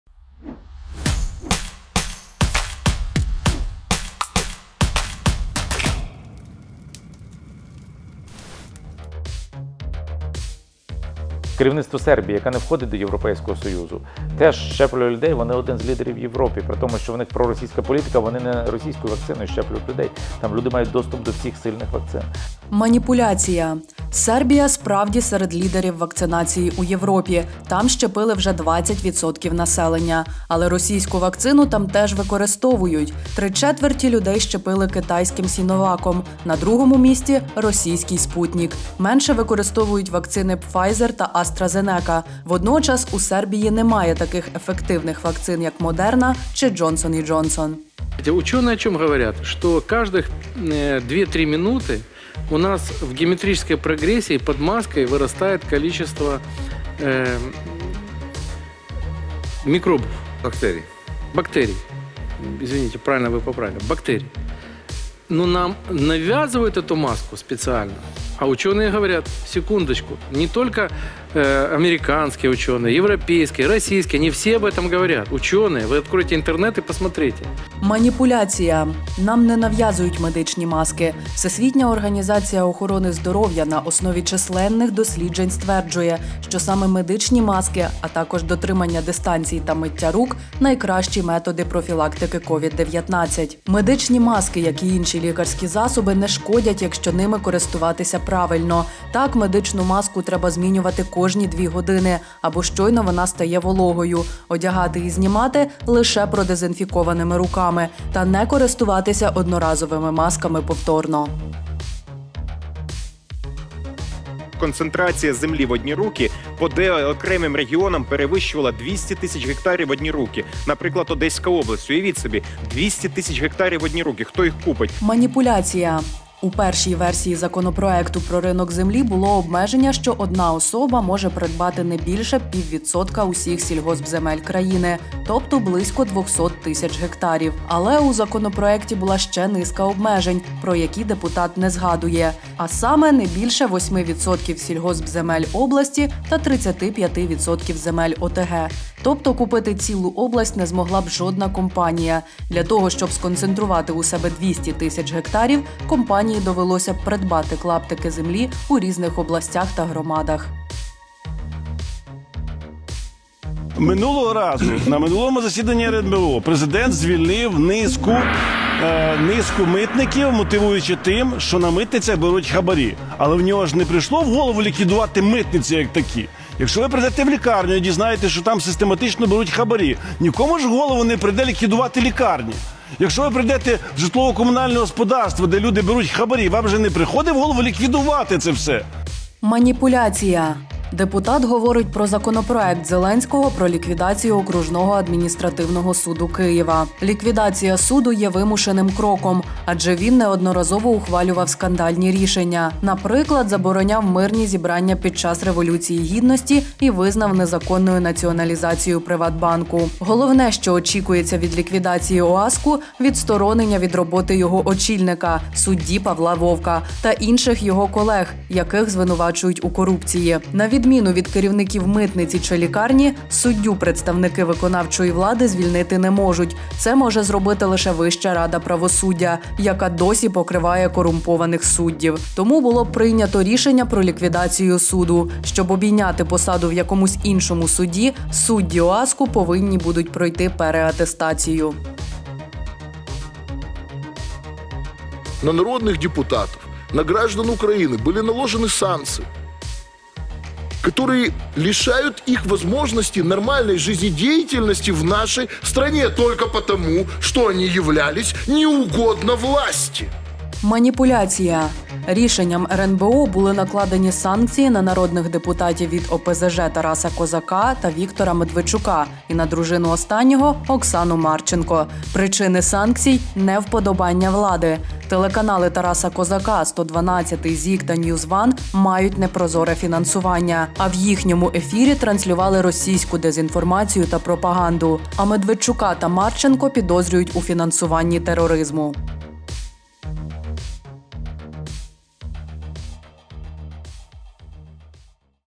Брехня від політиків. Княжицький, Качний, Колтунович, Рудик, Кива
00:08 — Микола Княжицький, депутат, «Європейська Солідарність»:
«Керівництво Сербії, яка не входить до Європейського Союзу, теж щеплює людей, вони один з лідерів у Європі. При тому, що в них проросійська політика, вони не російською вакциною щеплять людей, там люди мають доступ до всіх сильних вакцин».

00:48 — Олександр Качний, депутат, ОПЗЖ:
«Вчені про що говорять? Що кожні 2-3 хвилини у нас у геометричній прогресії під маскою зростає кількість бактерій. Але нам нав'язують цю маску спеціально. А вчені говорять: «Секундочку!». Не лише американські вчені, європейські, російські. Вони все про це говорять. Вчені! Ви відкрийте інтернет і подивіться».

01:52 — Олександр Колтунович, депутат, ОПЗЖ:
«Концентрація землі по окремих регіонах перевищувала 200 тис. га в одні руки (у першій редакції законопроекту про ринок землі - ред.). Наприклад, Одеська область, уявіть собі, 200 тис. га в одні руки, хто їх купить?».

02:41 — Сергій Рудик, депутатська група «За майбутнє»:
«Минулого разу, на минулому РНБО президент звільнив низку митників, мотивуючи це тим, що на митницях беруть хабарі. Але йому не прийшло в голову ліквідувати митниці як такі. Якщо ви прийдете в лікарню і дізнаєтеся, що там систематично беруть хабарі, нікому ж у голову не прийде ліквідувати лікарню. Якщо ви прийдете в житлово-комунальне господарство, де люди беруть хабарі, вам же не приходить в голову ліквідувати це все».

04:03 — Ілля Кива, депутат, ОПЗЖ:
«На народних депутатів, на громадян України були накладені санкції, які позбавляють їх можливості нормальної життєдіяльності у нашій країні. Тільки тому, що вони не подобаються владі».